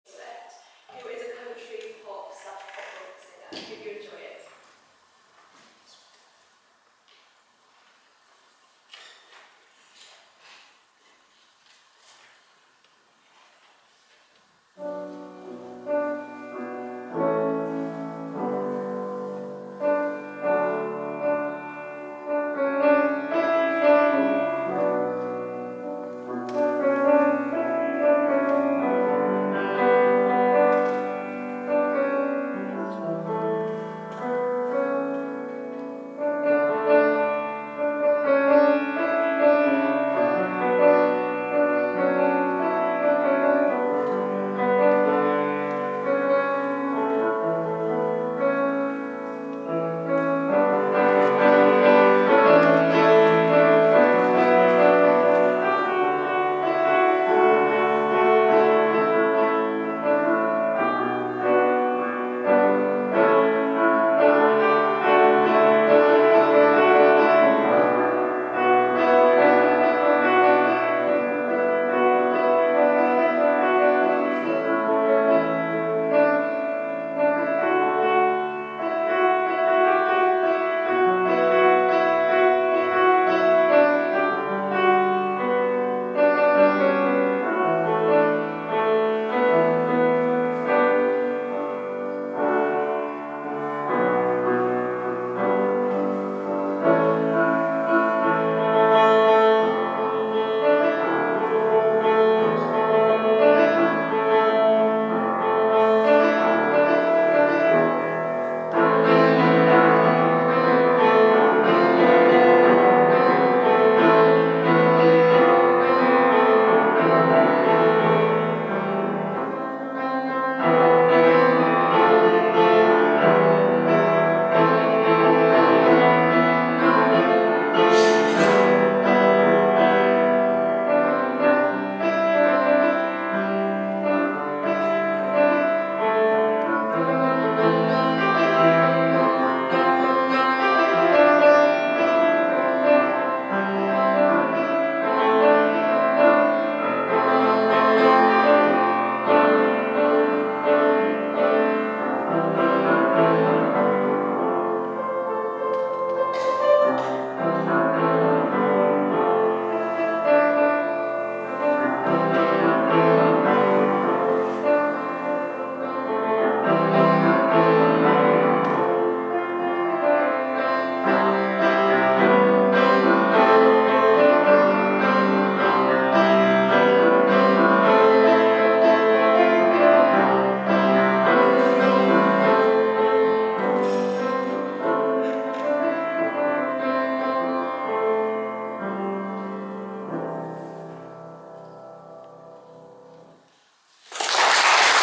Winchcombe Festival
Taylor Swift Medley arranged and played by Lilya